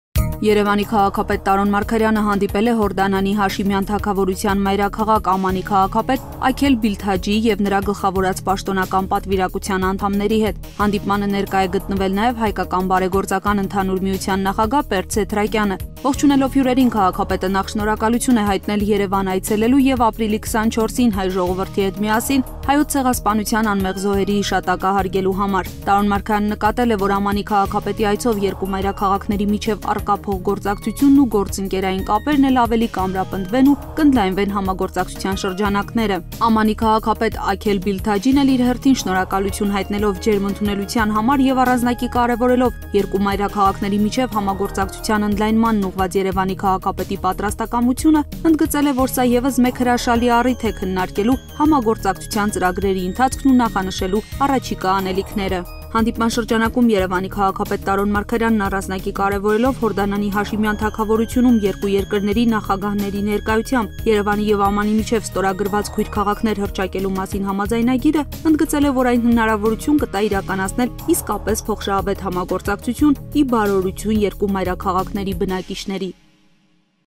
25.04.15 Տարոն Մարգարյանը հանդիպել է Ամմանի քաղաքապետի հետ
Ապրիլի 25-ին Երևանի քաղաքապետ Տարոն Մարգարյանը հանդիպել է Հորդանանի Հաշիմյան Թագավորության մայրաքաղաք Ամմանի քաղաքապետ Աքել Բիլթաջիի հետ, ով Երևան է այցելել Հայոց ցեղասպանության 100-ամյա տարելիցին նվիրված միջոցառումներին մասնակցելու համար։